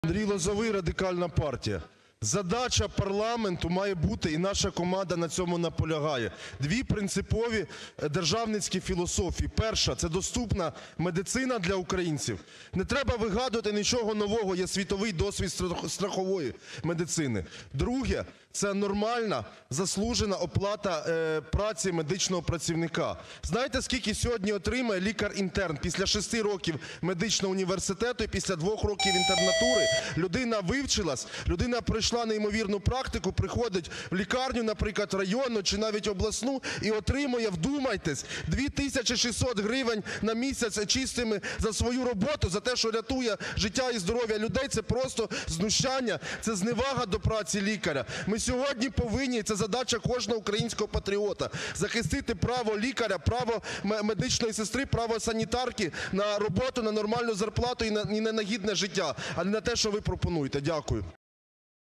Лозовий: Дайте медикам гідну зарплату!
Андрій Лозовий під час розгляду так званої медичної реформи заявив, що зарплата лікарів принизливо маленька. Політик наголосив, що влада повинна закласти у реформу підвищення зарплати лікарів і зробити послуги більш доступними.